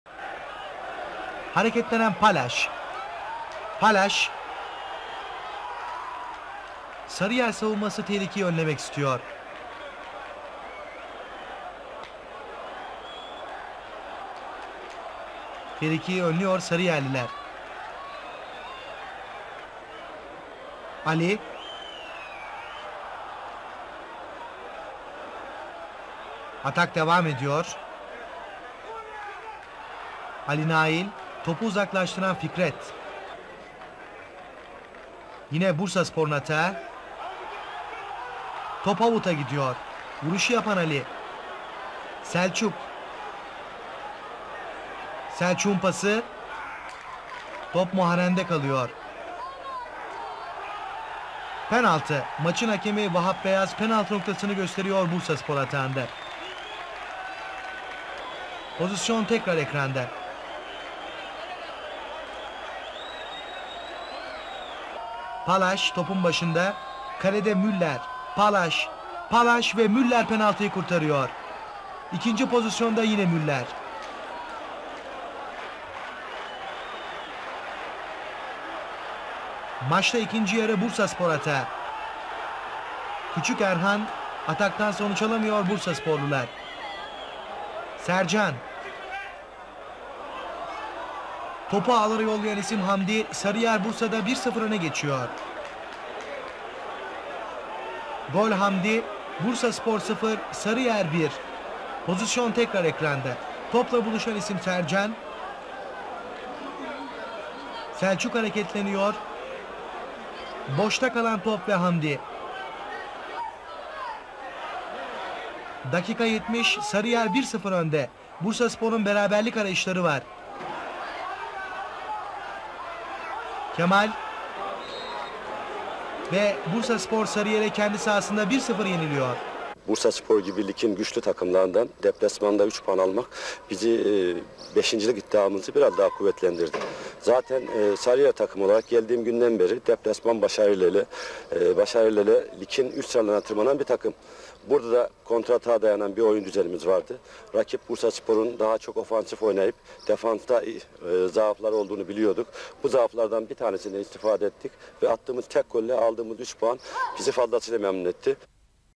1989 1990 Bursaspor Sarıyer PALAZS MÜLLER'İ GEÇEMEDİ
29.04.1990
bursaspor: muharrem kayan, ersel uzgur, yalçın gündüz, ahmet suphi evke, andrzej palasz, tuncay toper, taner ertaş, ali nail durmuş, ali beykoz, erhan kiremitçi, kemal şahinoğlu (dk. 74 hulusi bozkurt)

teknik direktör: yılmaz vural

sarıyer: metin mert, feridun alkan, cem pamiroğlu, mehmet kaplan, esat bayram, osman yıldırım, hamdi çam (dk. 82 tekin çelik), fikret demirer, selçuk yula (dk. 72 engin ülker), cengiz güzeltepe, sercan görgülü

teknik direktör: ahmet suat özyazıcı

gol:
(0-1) hamdi çam dk. 69

sarı kartlar:
yalçın gündüz dk. 43 (bursaspor)
cengiz güzeltepe dk. 37, hamdi çam dk. 65, metin mert dk. 84 (sarıyer)